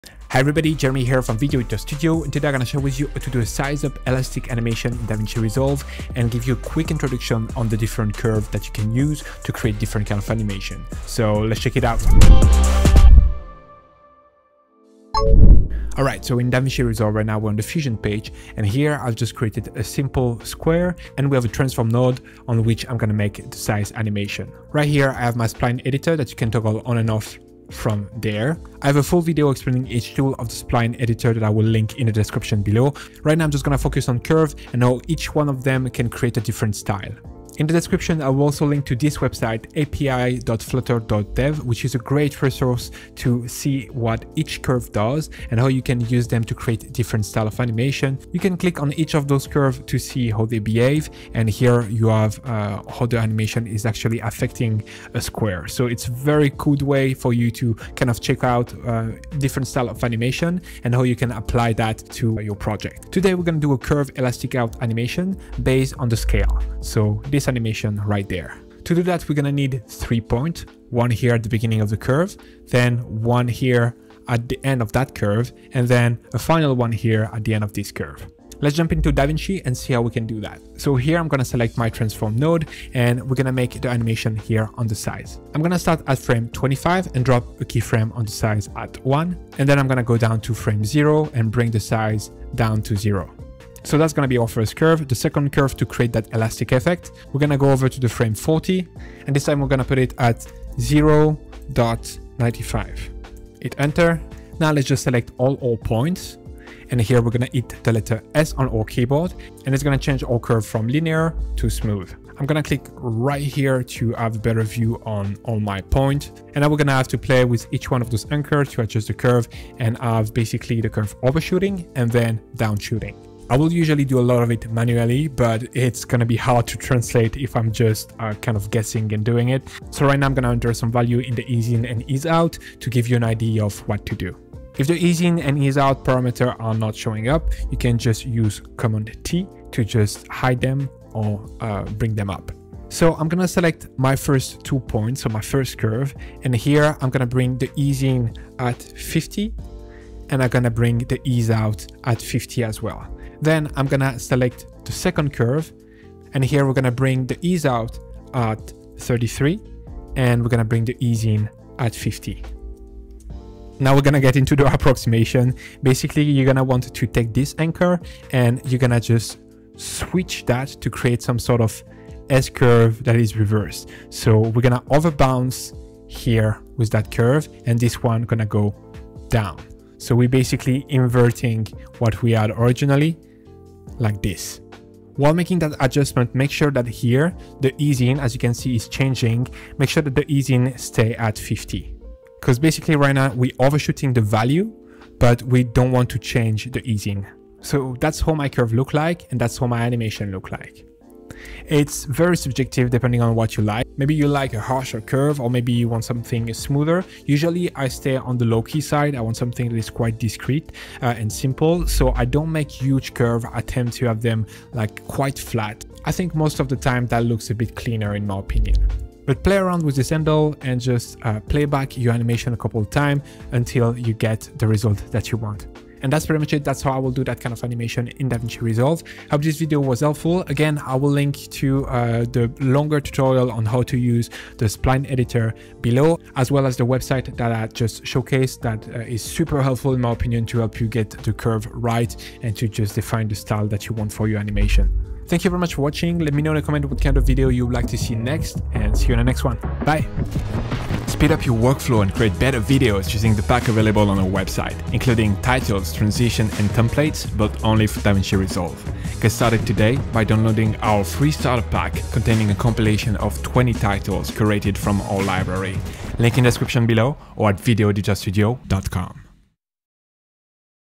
Elastic Curve Animation in Davinci Resolve
In this Davinci resolve tutorial, I'll share with you how to do an elastic bounce curve animation and share with you some tips to get smoother animations.